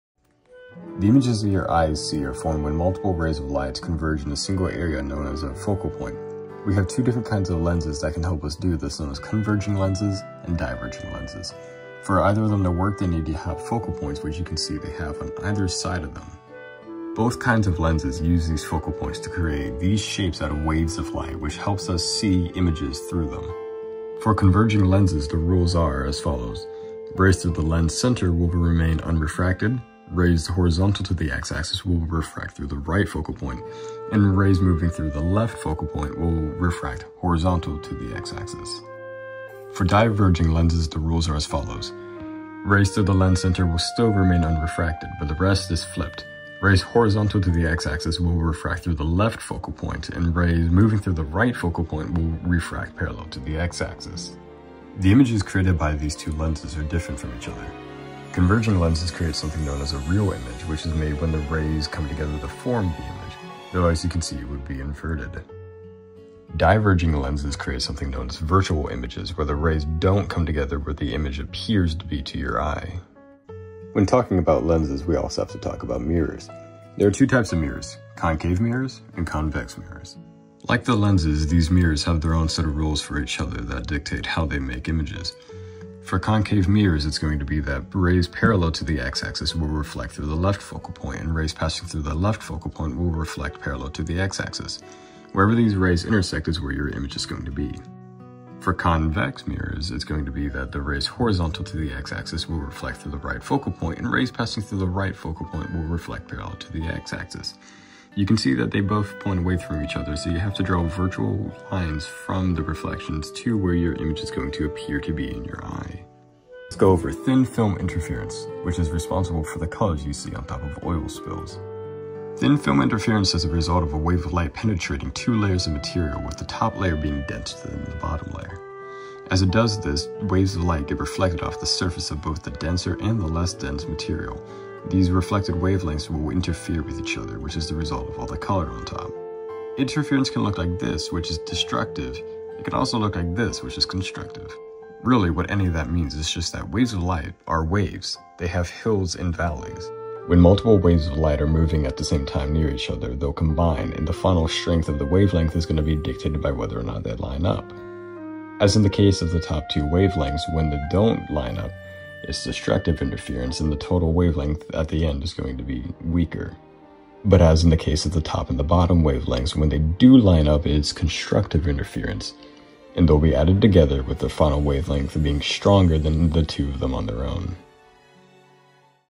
Crash Course in Everything Lenses, Mirrors, and Thin-Films
Don't know the difference between multiple lenses? Can't wrap your head around how mirrors work? Ever wondered why oil spills have rainbows on their surface? Here's everything you need to know about optics ot better understand the world around you!